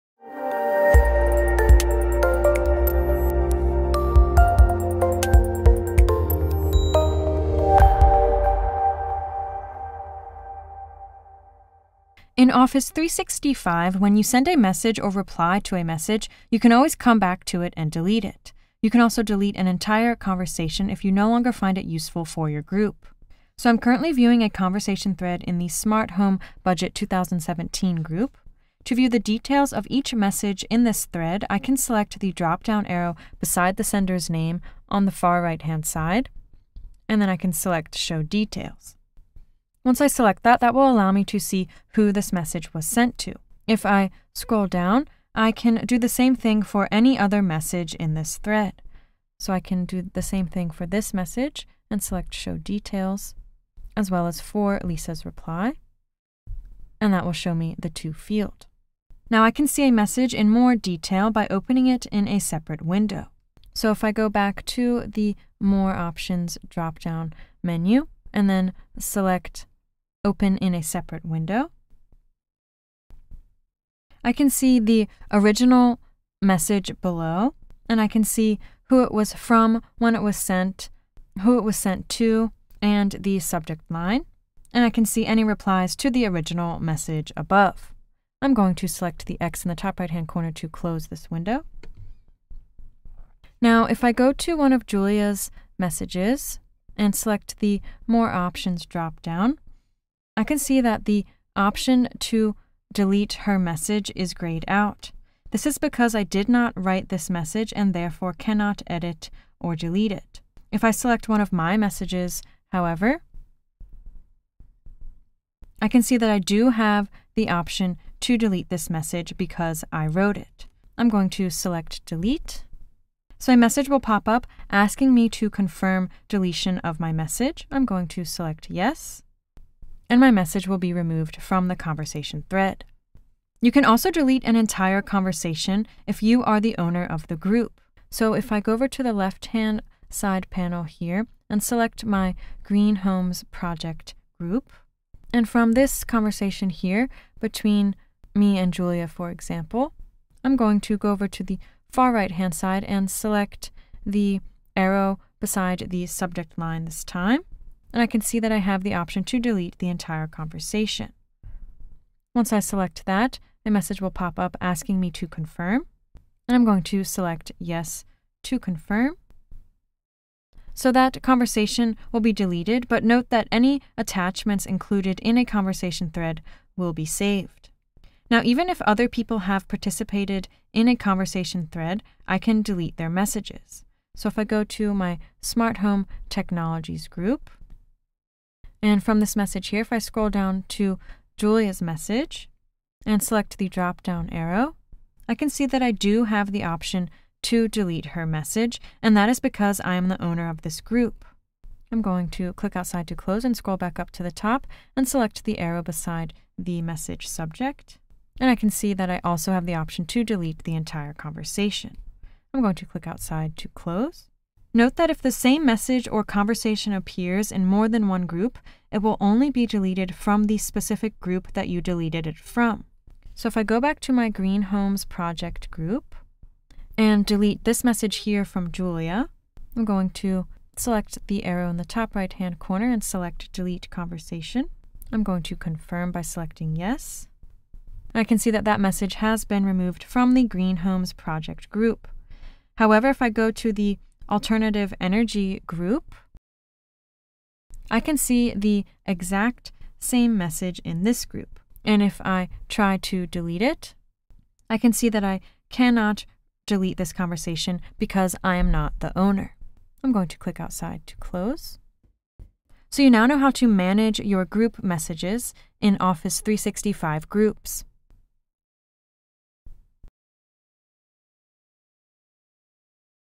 Managing your messages in Office 365
In Office 365, when you send a message or reply to a message, you can always come back to it and delete it. You can also delete an entire conversation if you no longer find it useful for your group.